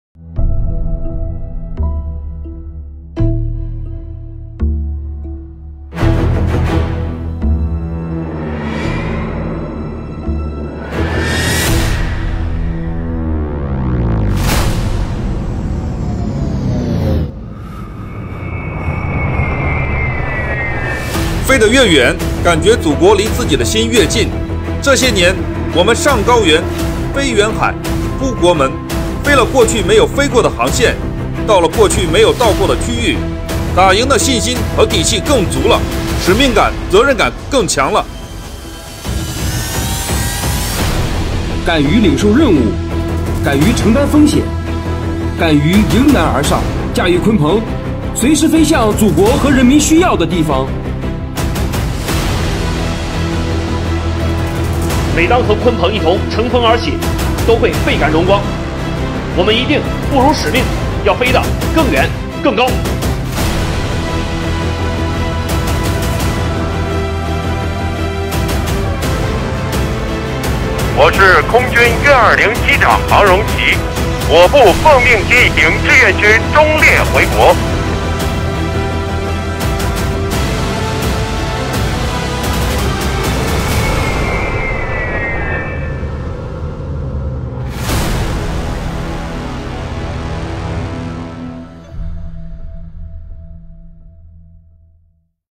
Máy bay vận tải hạng nặng Y-20 Không quân Chiến khu Trung ương Kỷ niệm 11 năm chuyến bay đầu tiên
Máy bay vận tải hạng nặng Y-20 Không quân Chiến khu Trung ương Trung Quốc Kỷ niệm 11 năm chuyến bay đầu tiên.

Source image in video: PLA Central Air Force